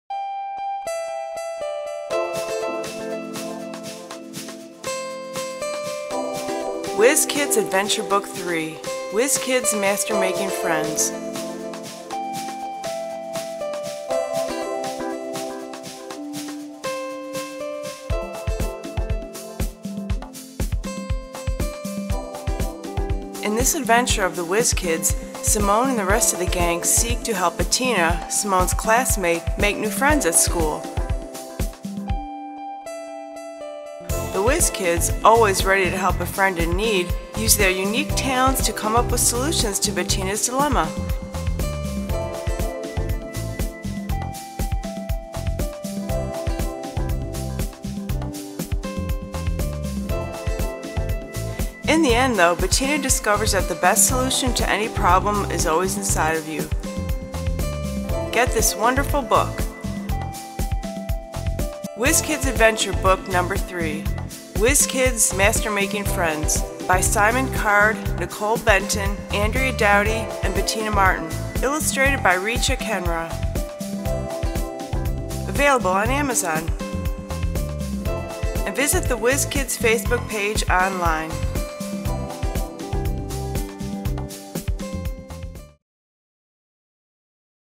WHIZ KIDZ MASTER MAKING FRIENDS Book Preview
Simone and the rest of the gang seek to help Bettina, Simone's classmate, make new friends at school.